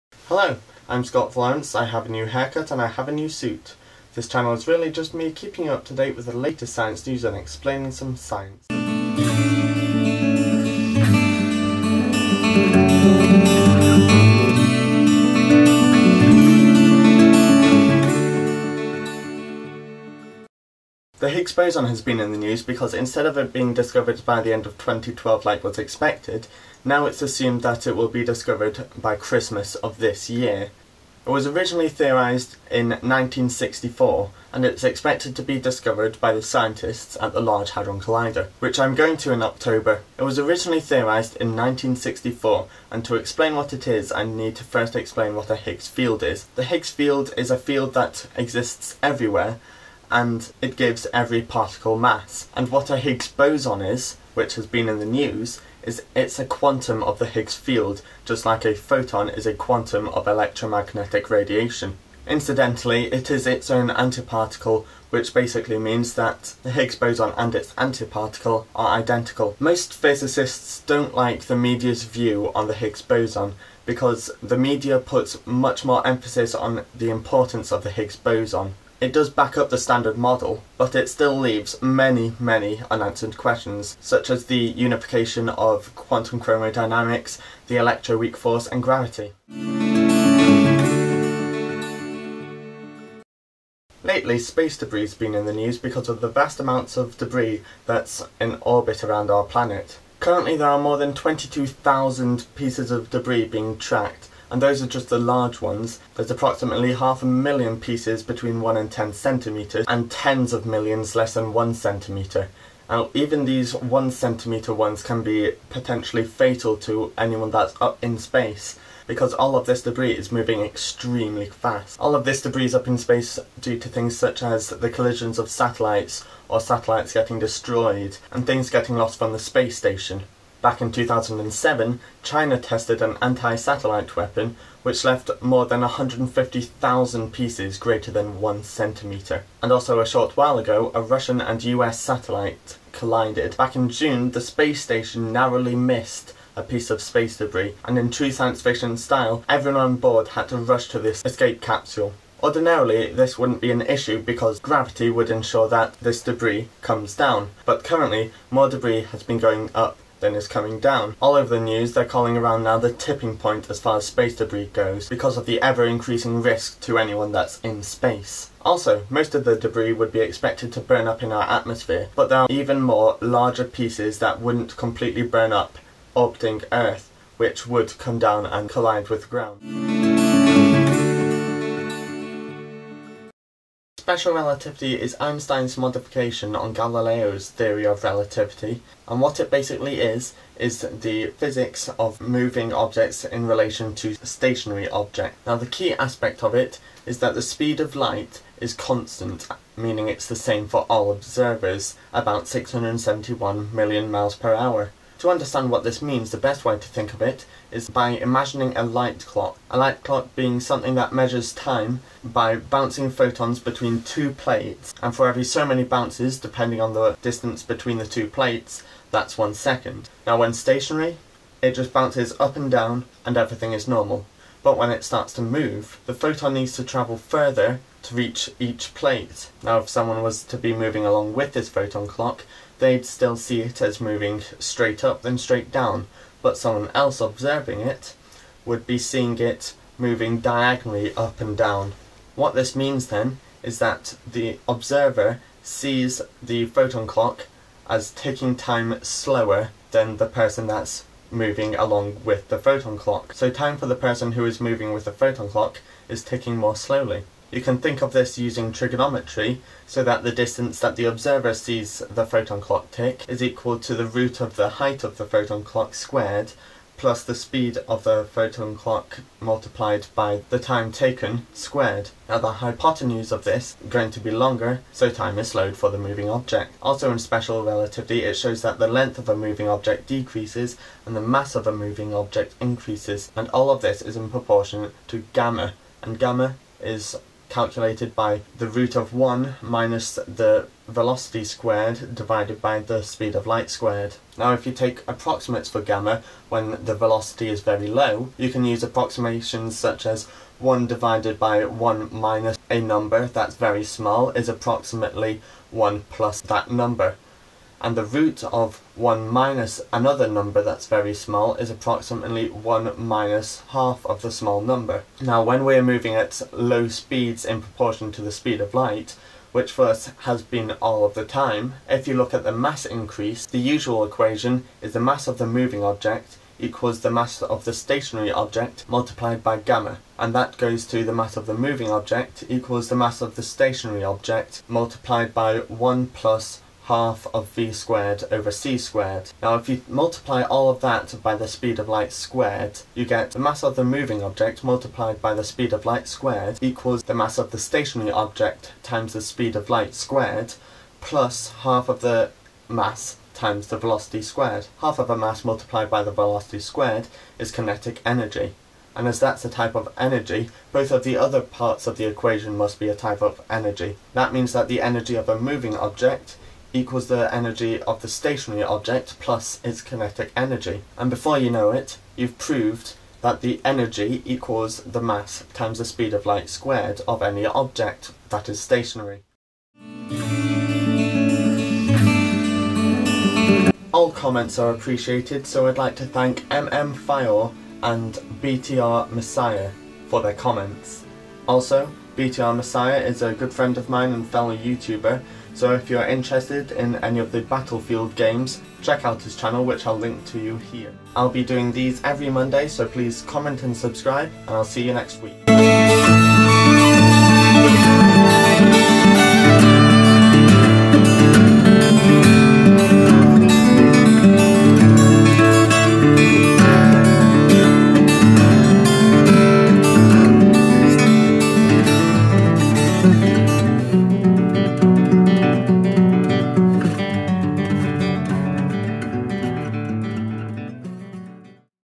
Higgs Boson, Space debris, Special relativity and proof E=mc 2
I talk about the latest science news, about the Higgs Boson, Dangers of space debris, Basics of special relativity and proof of how E=mc2.